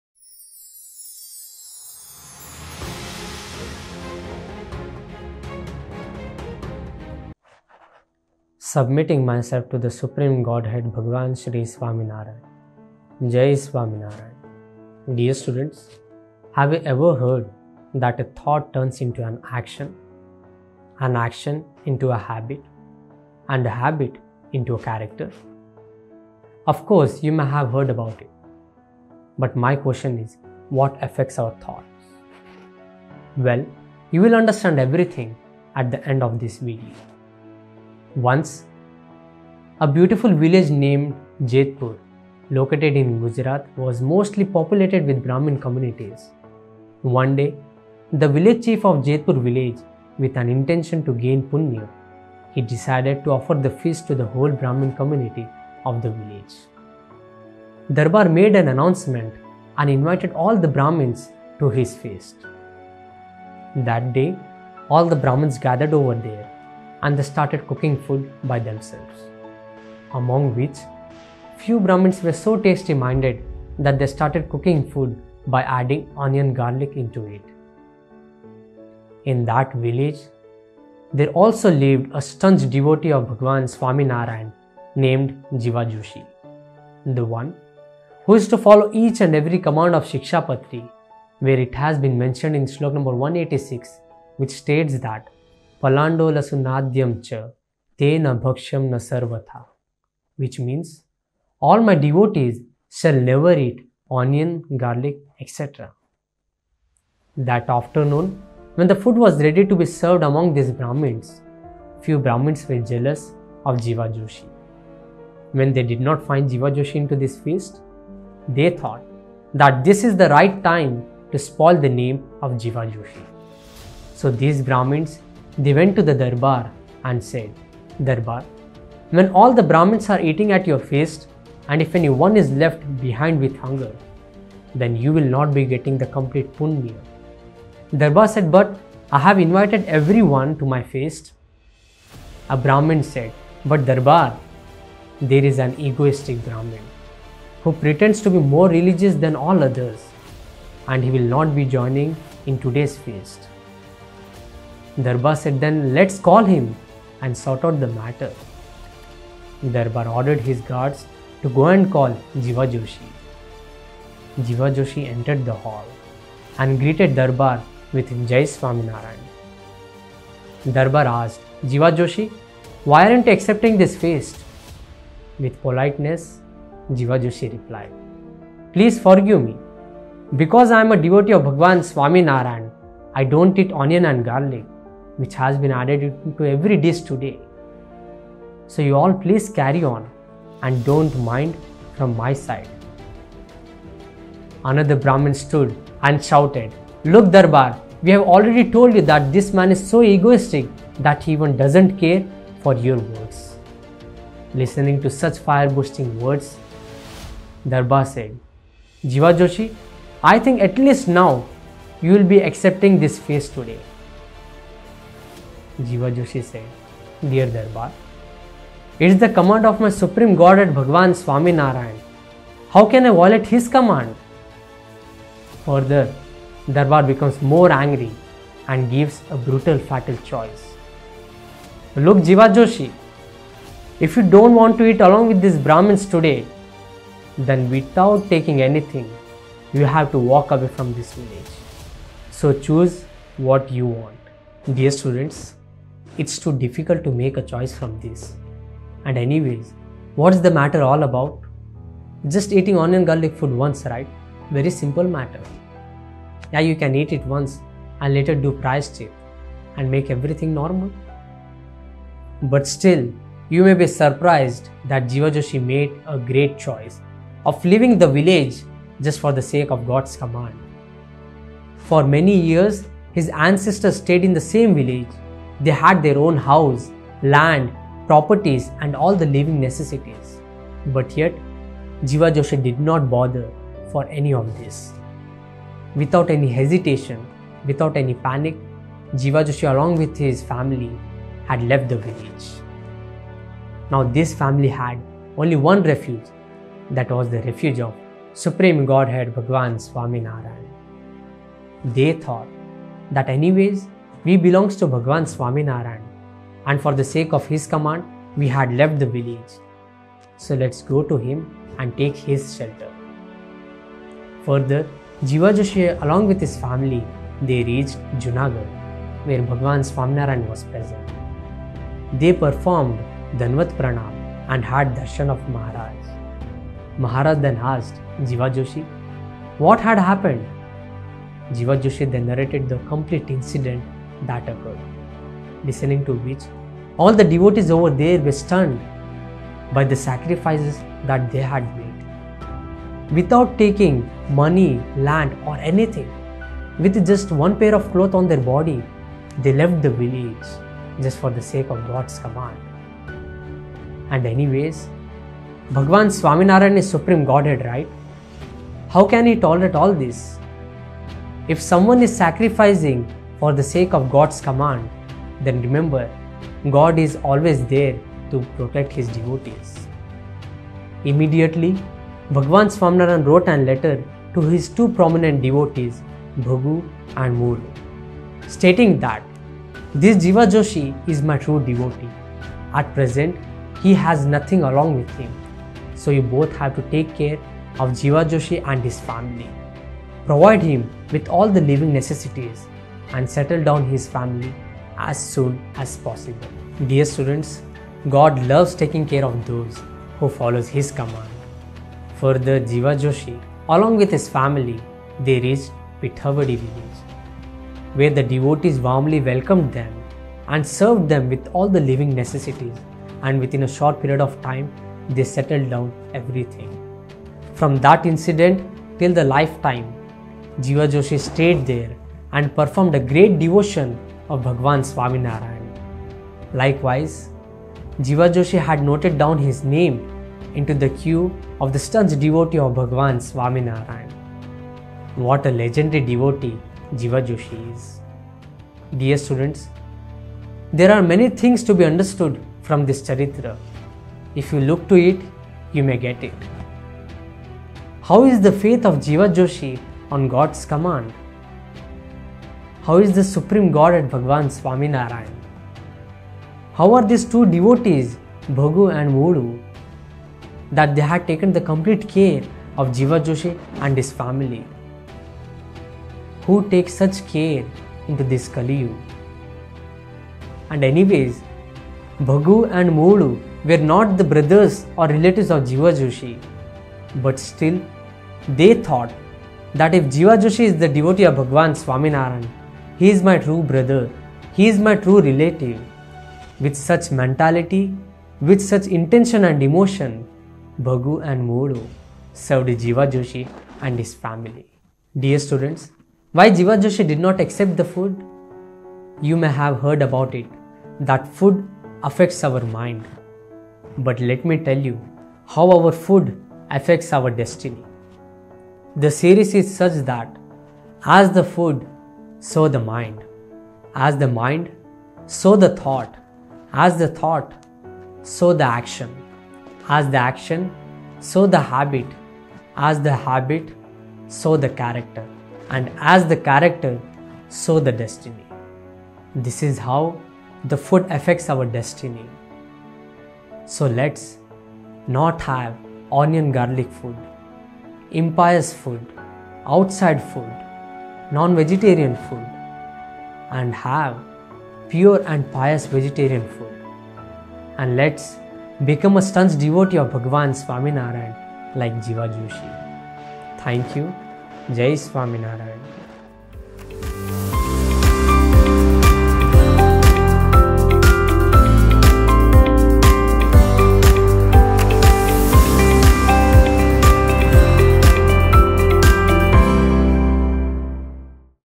Onion Garlic vs God's Command | Panch Vartman 5 | Shree Swaminarayan Gurukul Hyderabad
Supreme Godhead Bhagwan Shree Swaminarayan had given the Five Spiritual Disciplines for a householder devotee commonly known as Panch Vartman.
Among the five spiritual disciplines let’s go ahead with the Fifth vartaman which is 05. Not to Eat Impose food.

Have you ever heard that thought turns into action & action into habit & habit into character?
Of Course, you may have heard it, but my question is: What affects our thoughts?
Well, you will understand at the end of this video!

“As the food, so the mind; as the mind, so the thought; as the thought, so the action; as the action, so the habit; as the habit, so the character; as the character, so the destiny.”